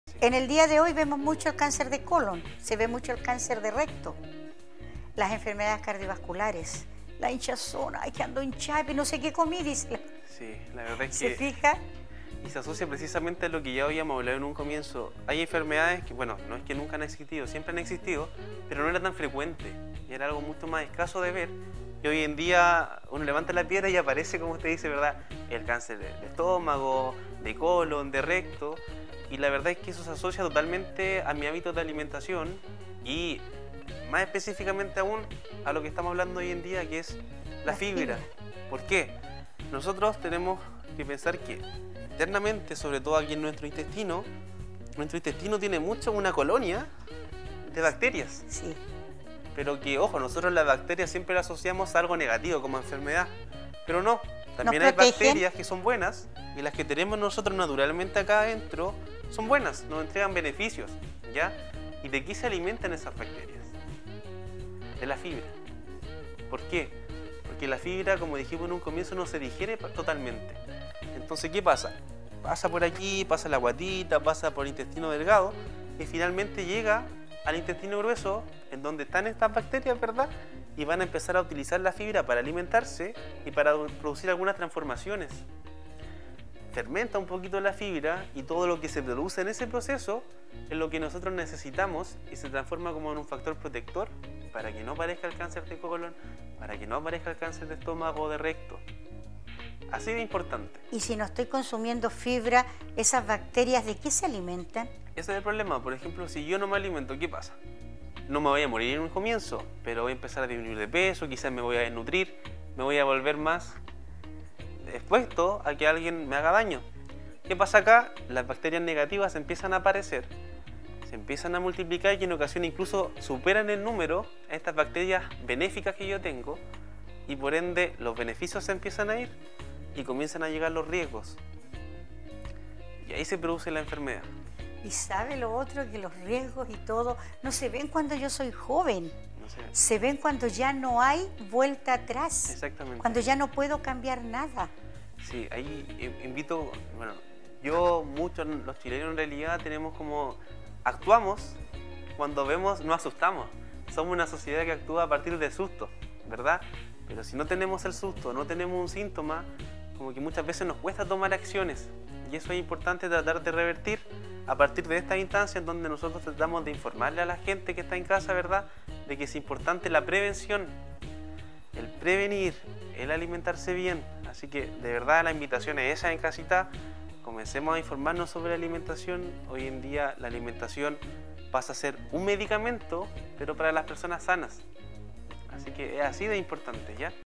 La fibra es de consumo diario: sin ella, se puede dar paso a graves enfermedades
Muchos reportajes en los medios de comunicación hablan de los "superalimentos" que en la realidad: han estado desde siempre. Debido a esto, el consumo de ciertos nutrientes y compuestos esenciales para una buena digestión suele considerarse como una "moda alimenticia" que en una semana puede regular el organismo, siendo que su uso debiese ser diario.

Es el caso de la fibra que se encuentra presente en variados alimentos, desmintiendo así la relación hecha con la prohibición de comidas que no estén bajo la categoría de "light".

Junto al nutricionista del hospital de San Vicente de Tagua Tagua, Miguel Yáñez, vemos los beneficios de su consumo, y que enfermedades o problemas puede desencadenar su bajo o nulo consumo, el cual también debe ir acompañado de agua, pues no solo se evita un irreparable daño al riñón, también es la combinación necesaria para que los beneficios de ambos funcionen a la perfección.